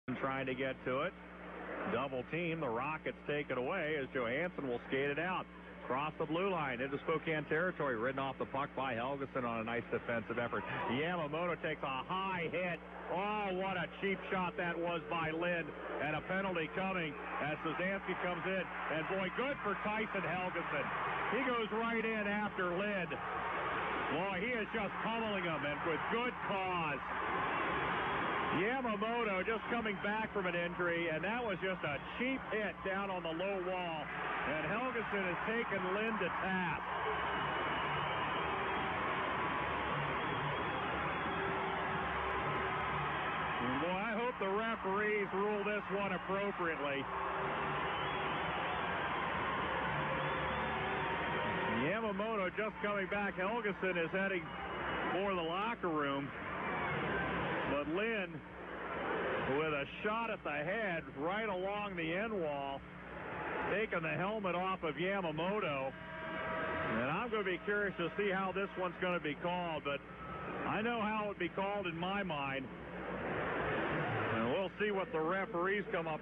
Kole Lind vs Tyson Helgesen Mar 12, 2016
Kole Lind vs Tyson Helgesen from the Kelowna Rockets at Spokane Chiefs game on Mar 12, 2016.